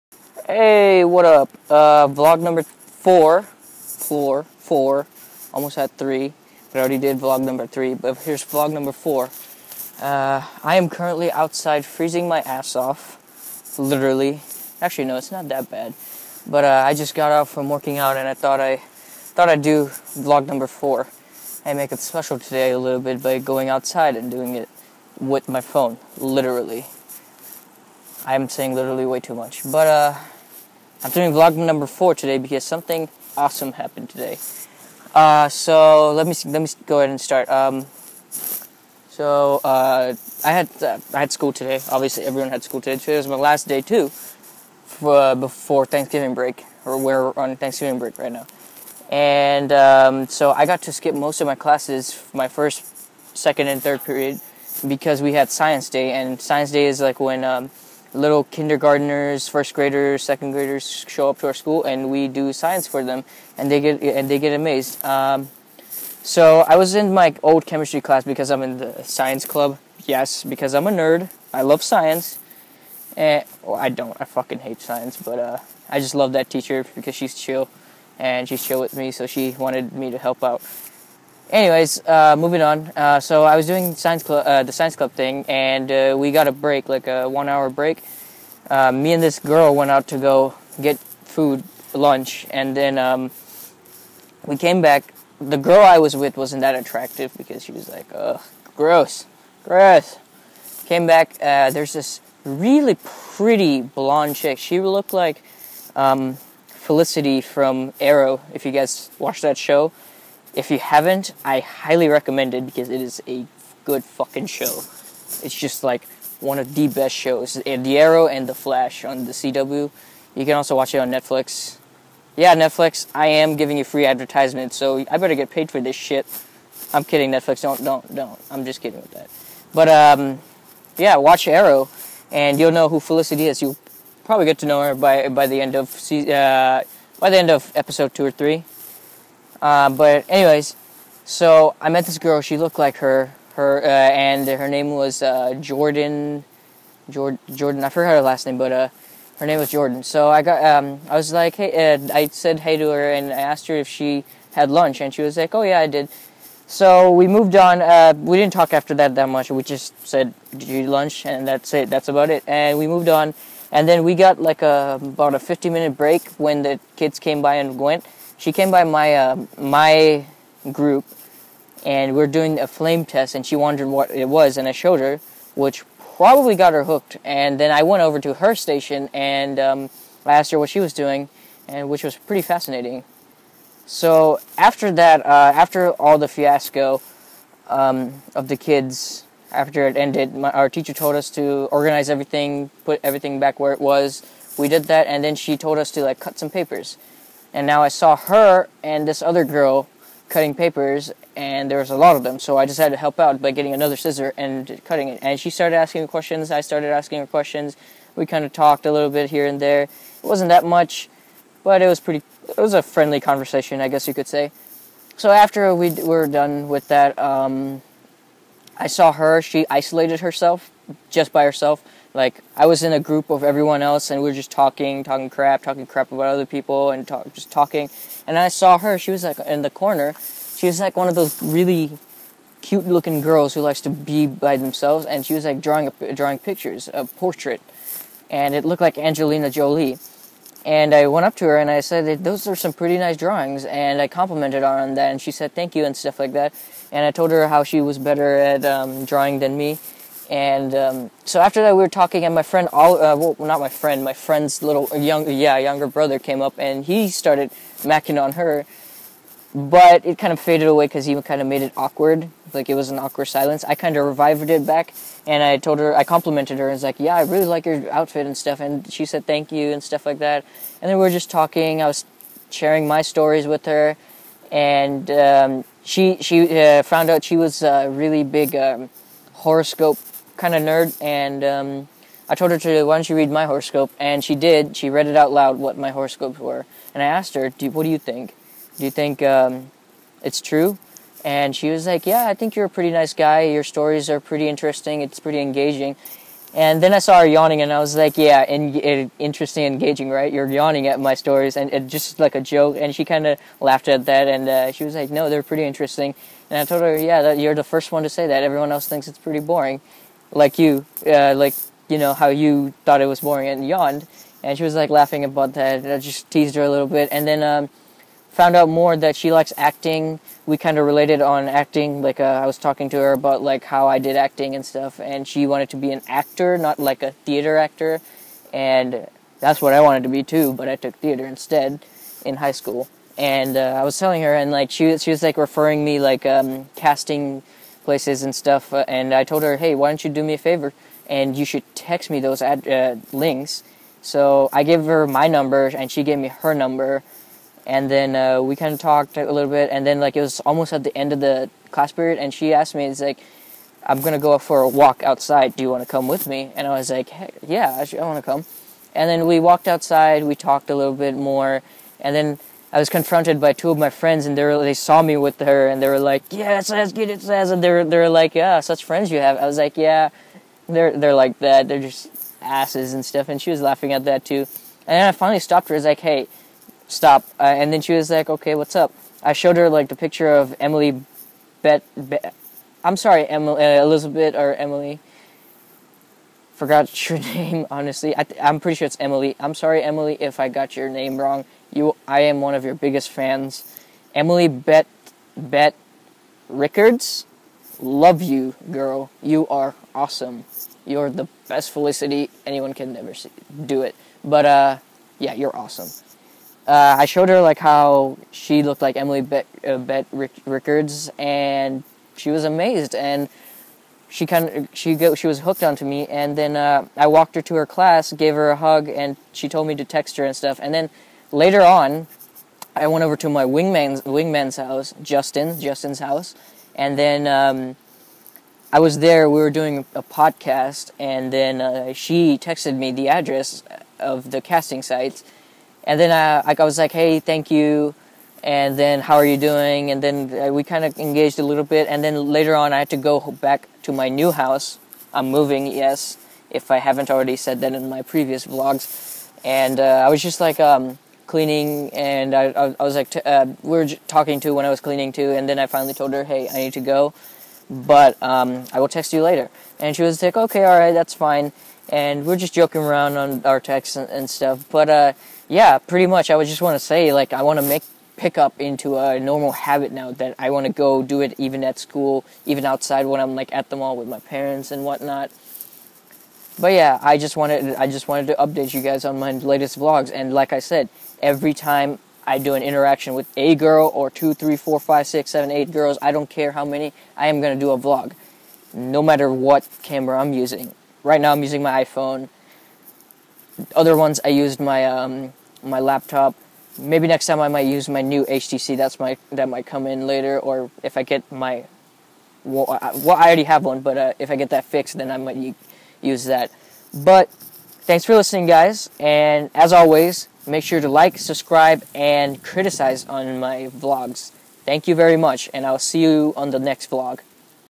Vlog #4 Why i'm obsessed with Emily Bett Rickards & Pickup!
Tonight, I explain why i'm so Obsessed with Emily, and how that reflects on my life! Also i explain why i want to Make Pickup as a Hobby.